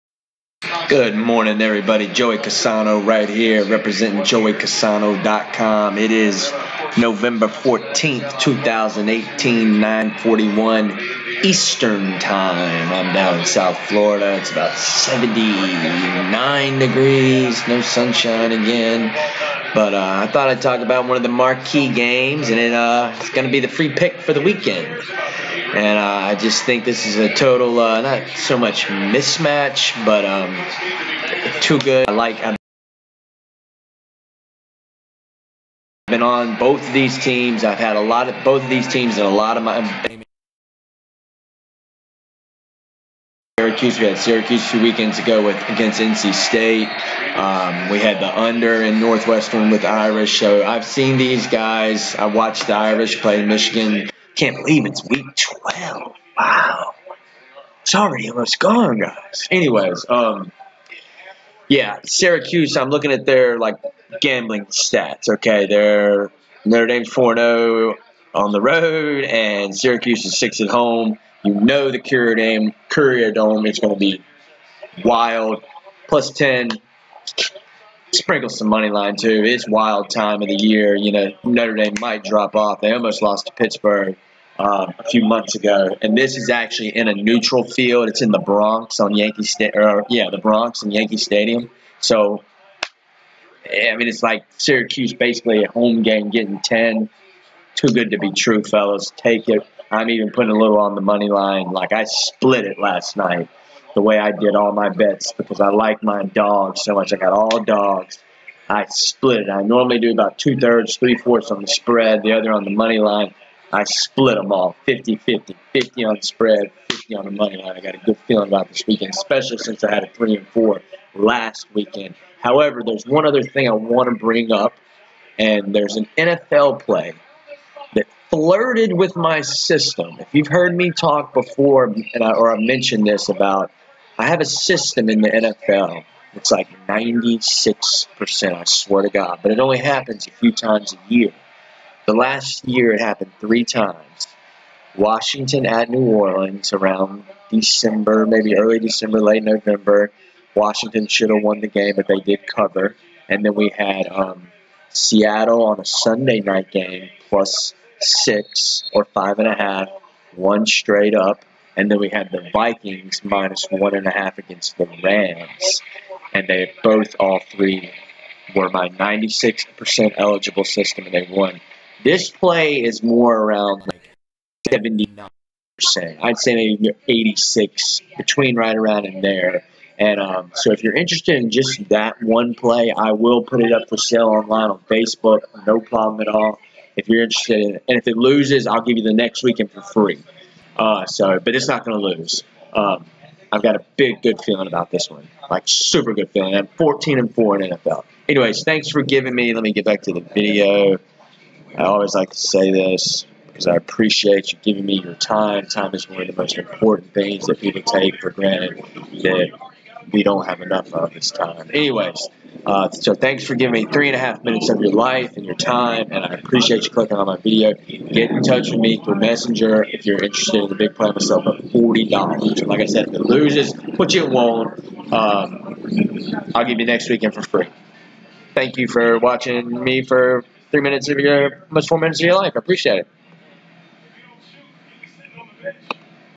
Syracuse Orangemen vs Nortre Dame Fighting Irish Week12 2018
TEXT: 281- WINNERS
Too many points for the Syracuse Orangemen to handle for the fighting Irish!